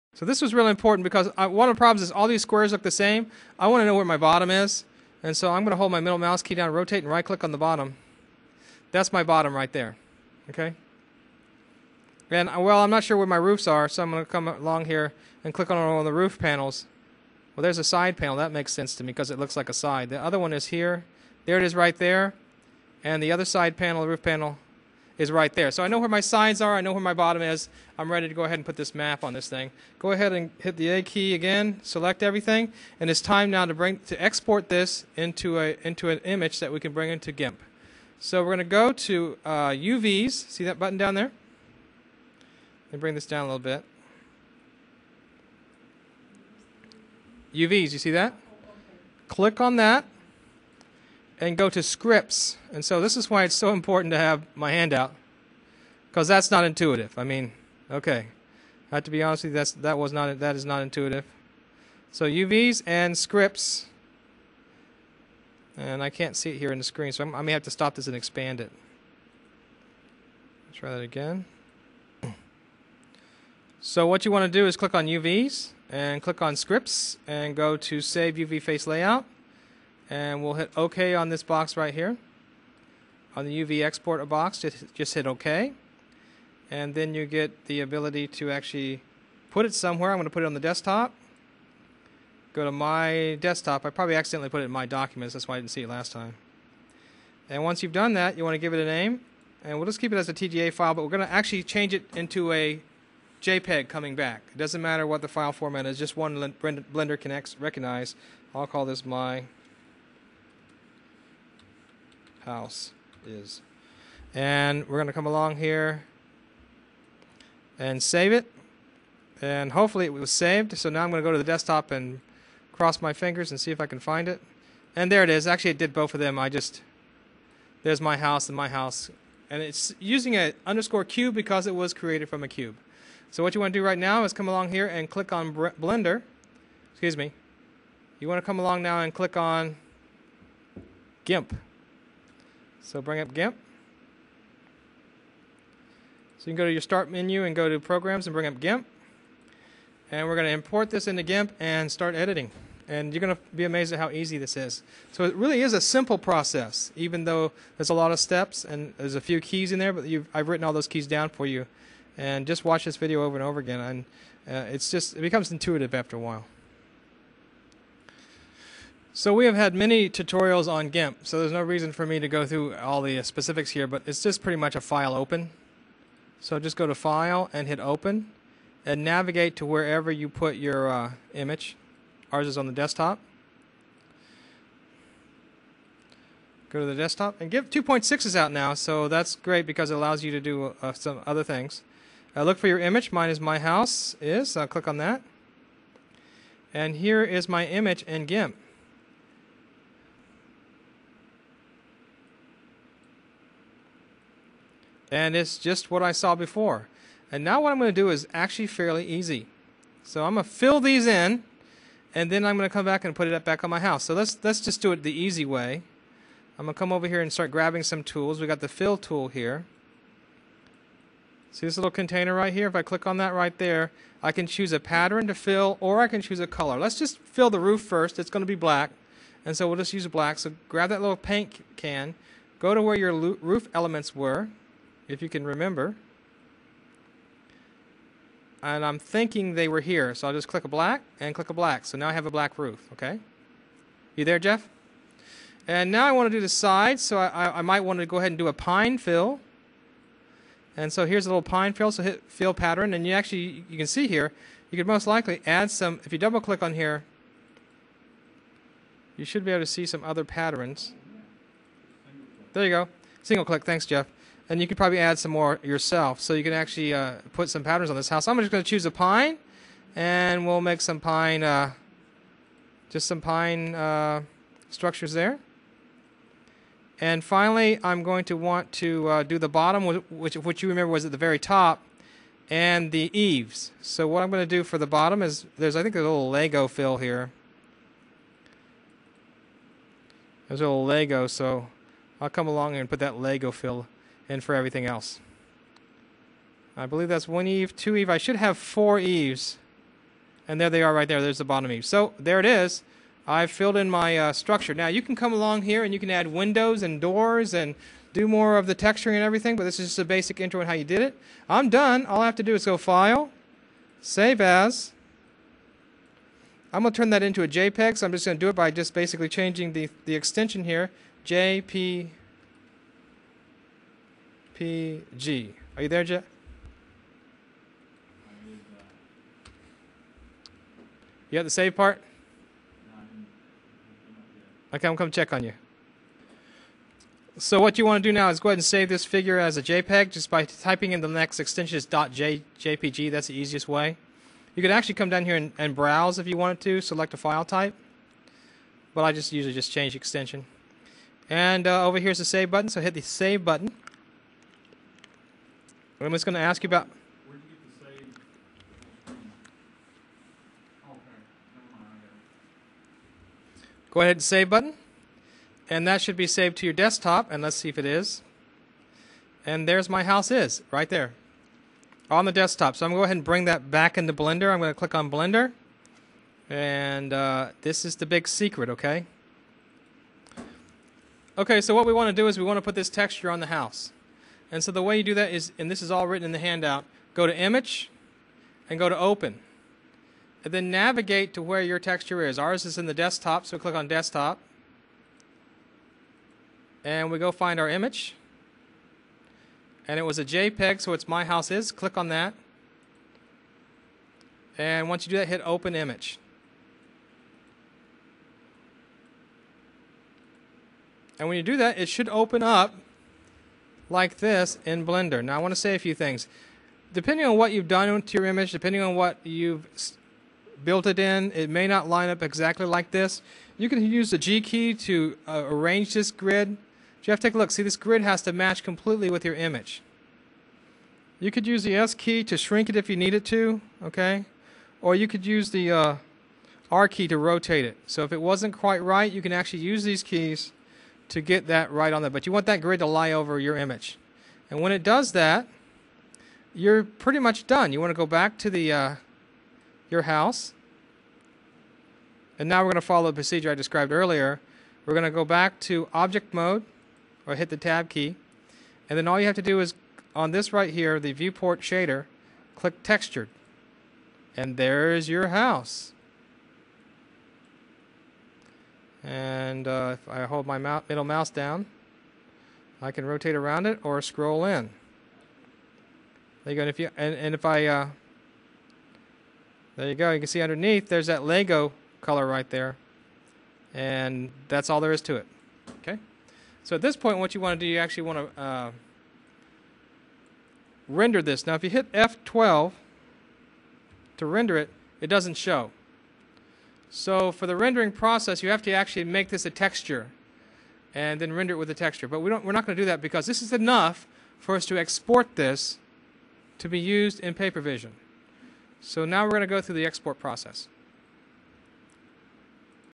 Blender UV Mapping Part 2
This is part 2 of UV Mapping in Blender. Here you learn to create your UV map in gimp and put it on your house.

This tutorial was created by Mike Lively of Northern Kentucky University.